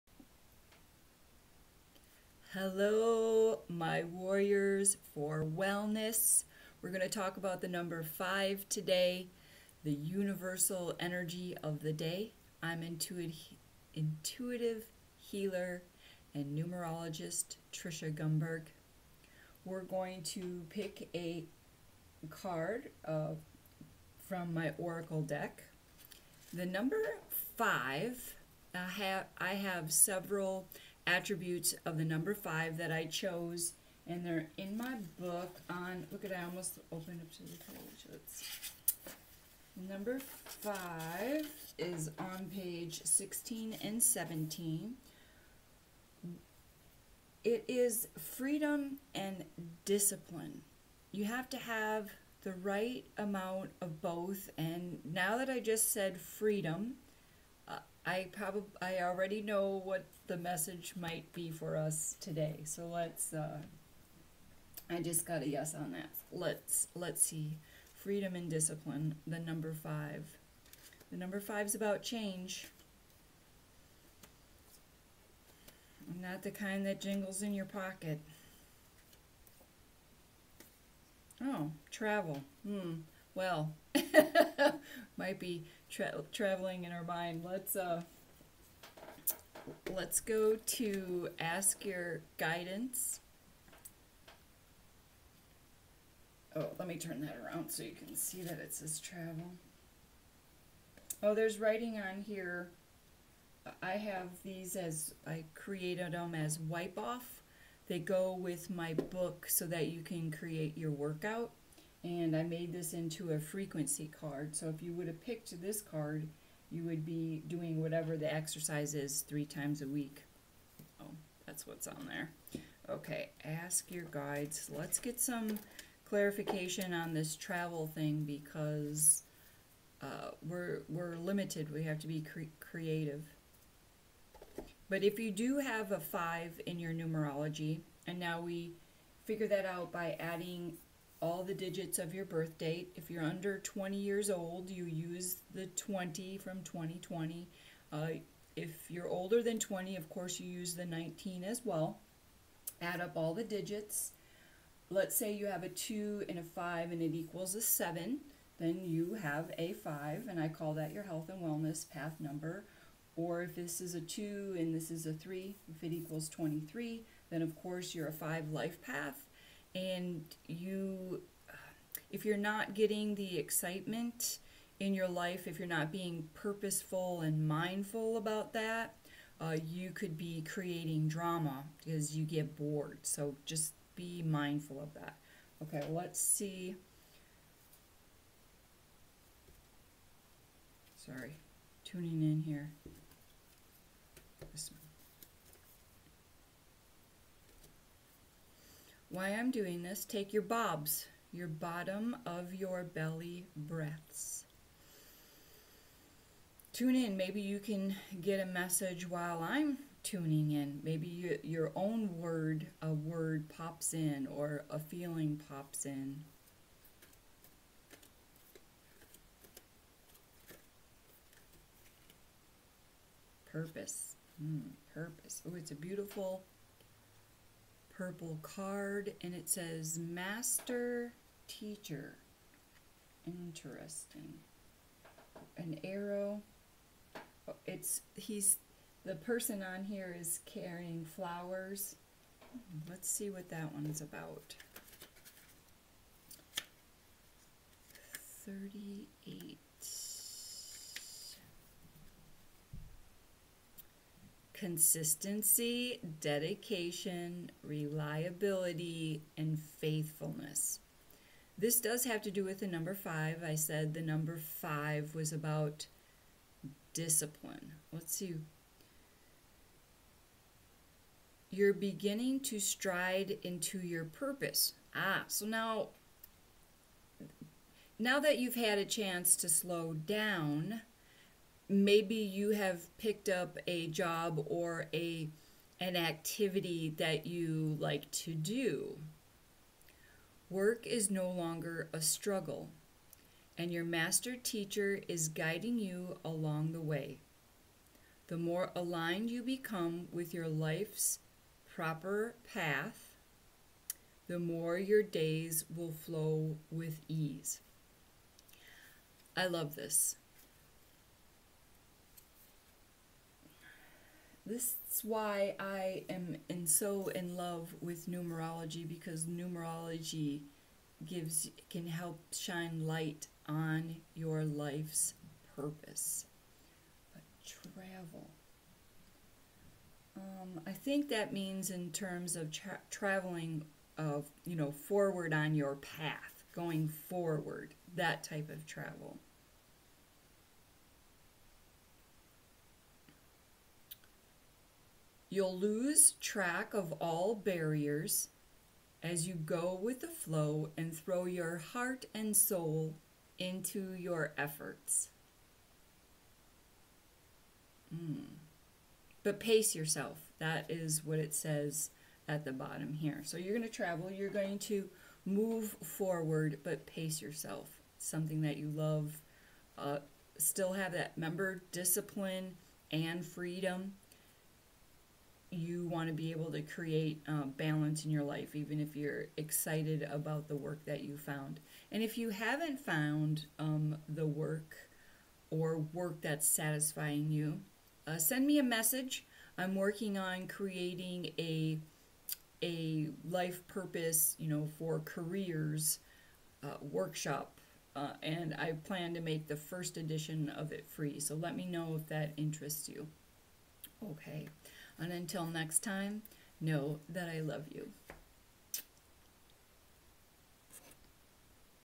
Daily Energy Readings 4/6/2020 (Monday)
Using Numerology and oracle cards, Intuitive Tricia Gunberg guides us.